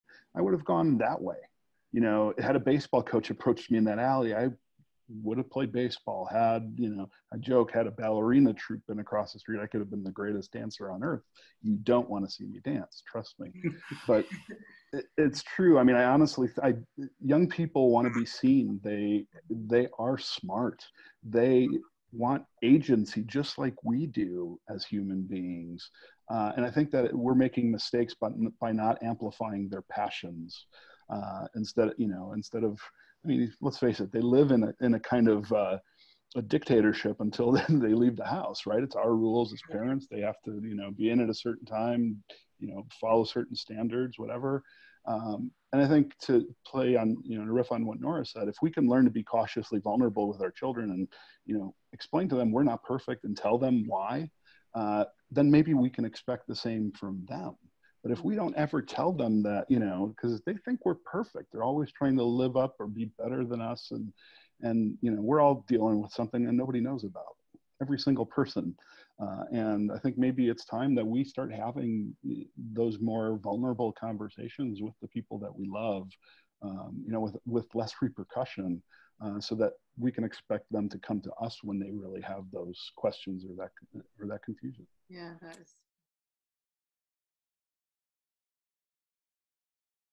Kids vulnerable to White nationalism can be differently directed w/ Christian Picciolini
"You know, had a baseball coach approached me in that alley, I would have played baseball. I joke, had a ballerina troupe been across the street, I could have been the greatest dancer on Earth. You don't want to see me dance. Trust me. But it's true.

"Young people want to be seen. They are smart. They want agency just like we do as human beings. And I think that we're making mistakes by not amplifying their passions. I mean let's face it, they live in a kind of a dictatorship until they leave the house. Right, it's our rules as parents. They have to be in at a certain time. You know, follow certain standards, whatever."